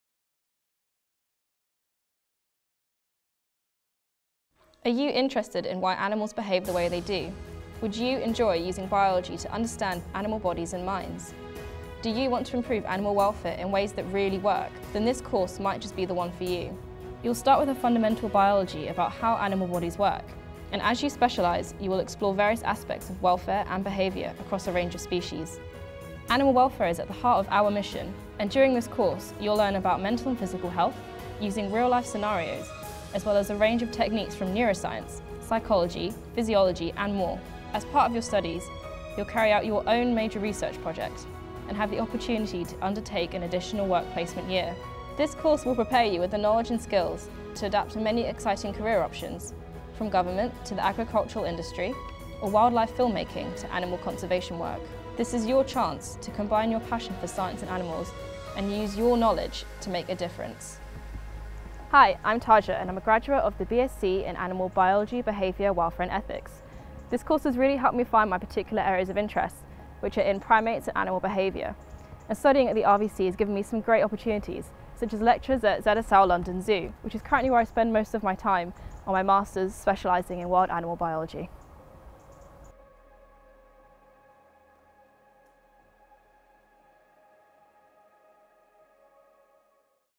BSc Animal Biology, Behaviour, Welfare & Ethics at the RVC
Animal welfare is at the heart of our mission, and it is a growing, dynamic field of study. The BSc Animal Biology, Behaviour, Welfare and Ethics builds on our international reputation in this field.

If you are passionate about animal welfare and wonder why different animals behave the way they do, then this course might be for you.

The course builds on the scientific excellence of our BSc Biological Sciences programme and is taught by staff from our outstanding Animal Welfare, Science and Ethics group. The group works closely with farmers, animal owners, policy makers and welfare charities.

During this programme, you will consider fundamental biological questions using a range of field techniques. These include animal perception and adaptation as well as exploring the practical and ethical implications of animal management and welfare. There will be opportunities to study welfare and behaviour across a range of species, including farm, companion, laboratory and zoo animals.

Part of the final year will be devoted to a major research project, to be carried out either at the RVC or with one of our partners.